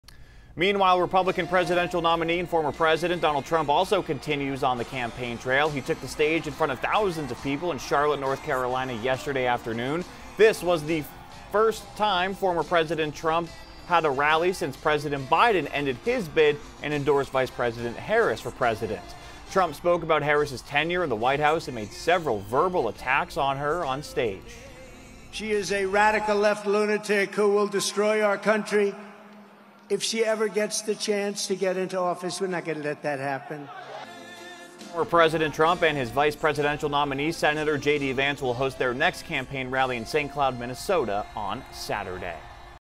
Trump rallies in North Carolina
Former President Trump and his vice presidential nominee J.D. Vance will host their next rally in Saint Cloud, Minnesota on Saturday.